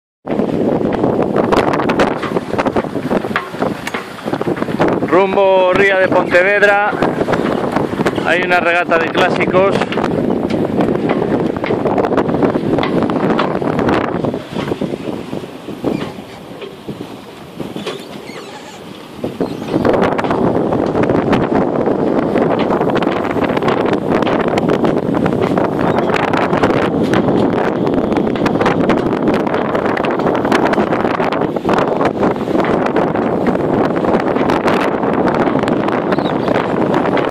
Navegando en las Rías Baixas con un Jeanneau Melody
El Mola Mola, Jeanneau Melody navegando rumbo a la Ría de Pontevedra